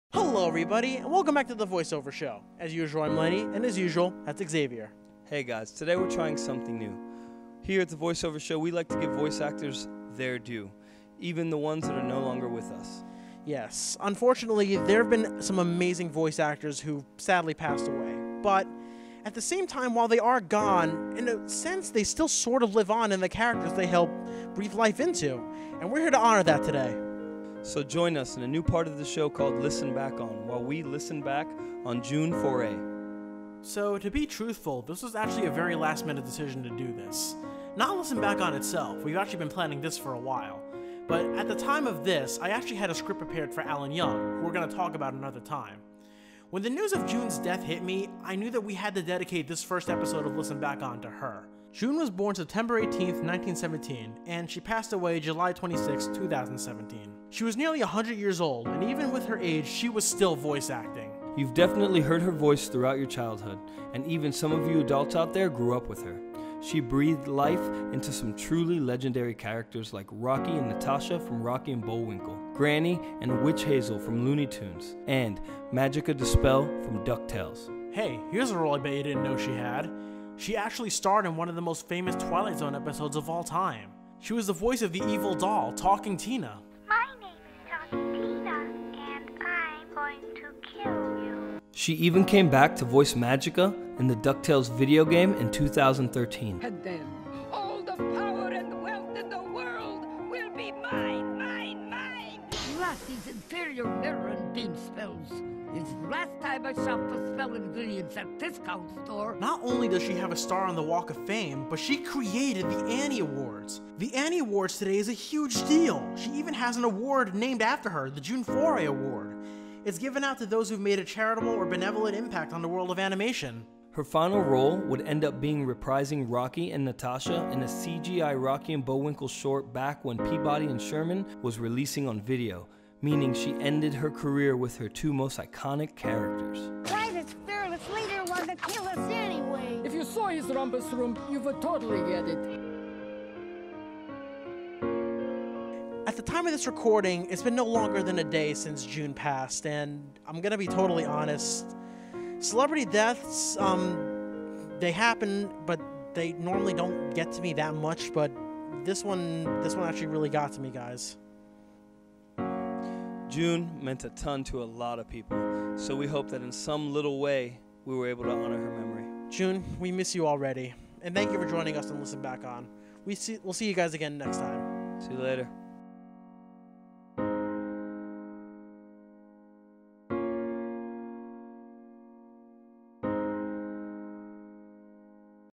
Listen Back On: June Foray | A Look At The Life of an Iconic Voice Actor
With the passing of iconic voice actor June Foray last week (July 26th 2017), Lenny and Xavier give a brief look at the career of the woman of a thousand voices. June is most known for her roles in Rockey & Bulwinkle, Ducktales, the Twilightzone, and Looney Tunes. We'll remember you always June!

FOLLOW TVOS ON TWITTER HERE!

LIKE THE OFFICIAL TVOS FACEBOOK PAGE!